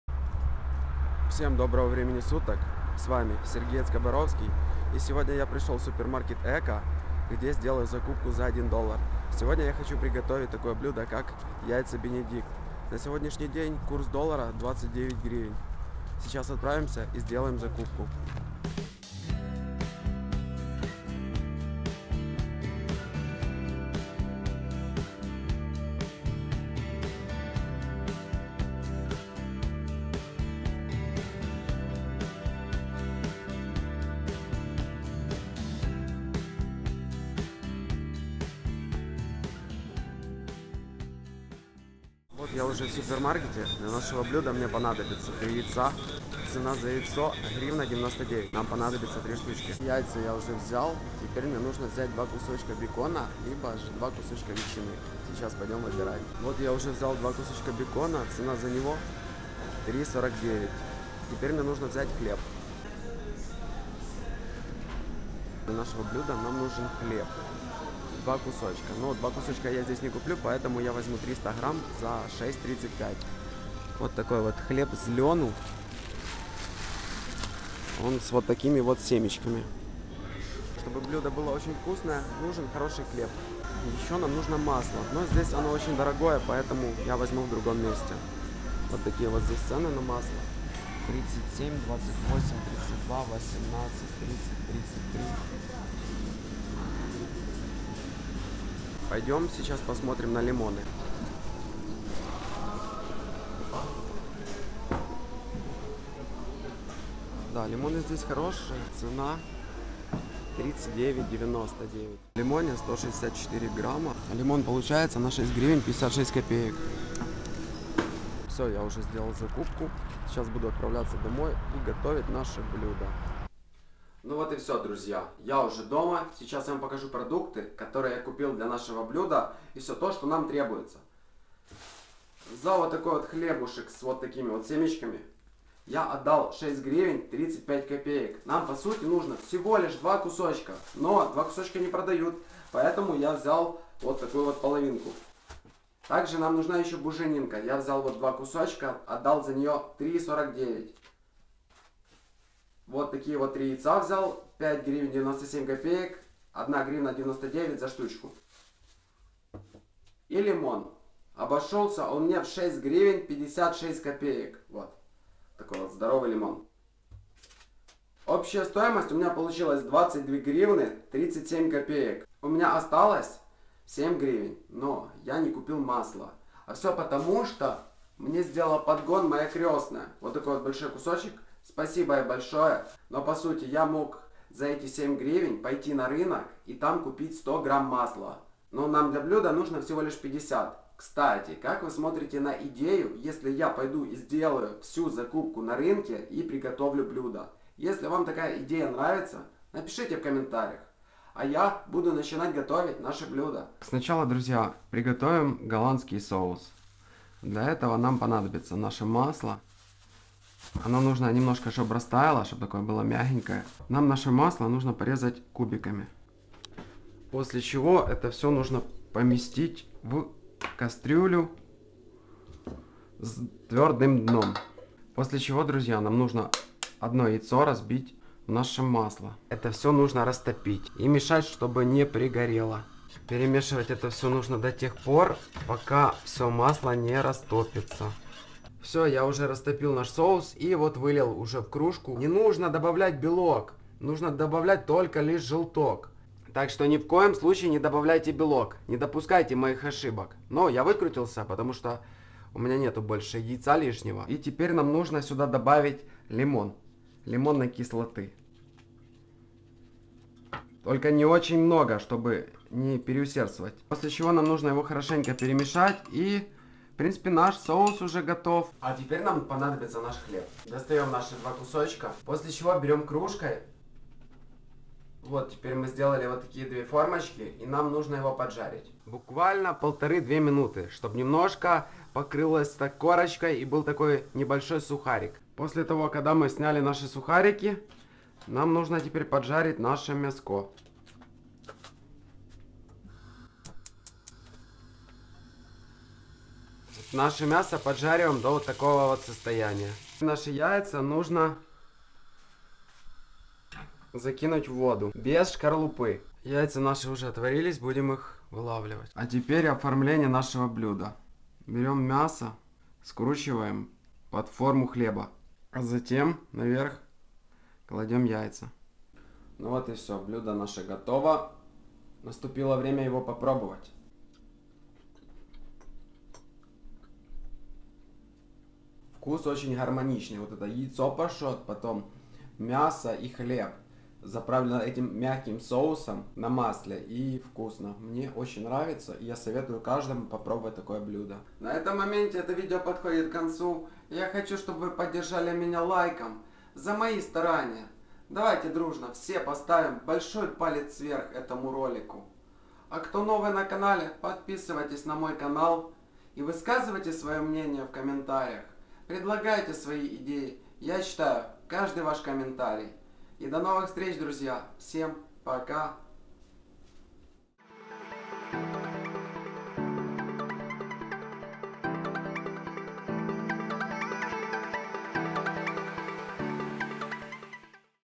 ОБЕД БОМЖА В УКРАИНЕ ЗА 1$ ДОЛЛАР !!! КАК ПРИГОТОВИТЬ БЕДНОМУ В КИЕВЕ ЯЙЦА БЕНЕДИКТ
Помогите деньгами на продукты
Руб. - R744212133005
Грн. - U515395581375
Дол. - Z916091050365